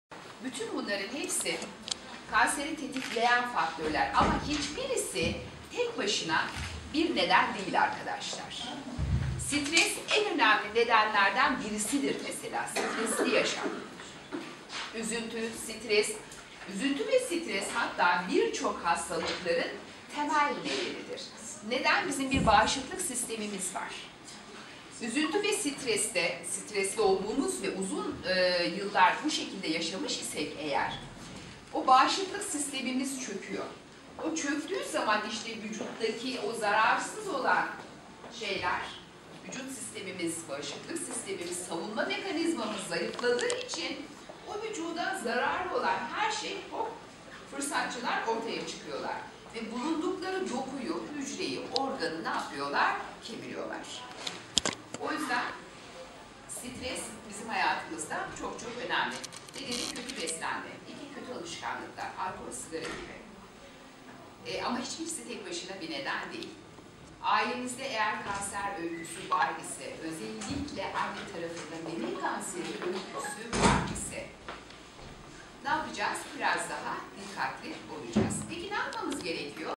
Can Hastanesi sağlık seminerlerine start verdi
AK Parti İlçe teşkilatı kadın kolları üyeleri ve AK Partili kadınlar Can Hastanesi Halkla İlişkiler Müdürü Güngör Bayrakdar’ın başlatmış olduğu sosyal sorumluluk projesi kapsamında meme sağlığı farkındalığı için bilgilendirme seminer düzenledi seminerde olası bir hastalık öncesi alınması gereken önemler ve erken teşhisin önemi Polatlı Özel Can Hastanesi Başhemşiresi Semiha Topsoy tarafından anlatıldı. Kendi kendine meme muayenesi de seminerde üzerinde durulan bir diğer önemli konu oldu. AK Parti İlçe Teşkilatı Kadın Kolları Başkanı Ayşegül Karaduman bu tarz sağlık seminerlerin belirli periyotlar ile düzenleneceğinin ifade etti.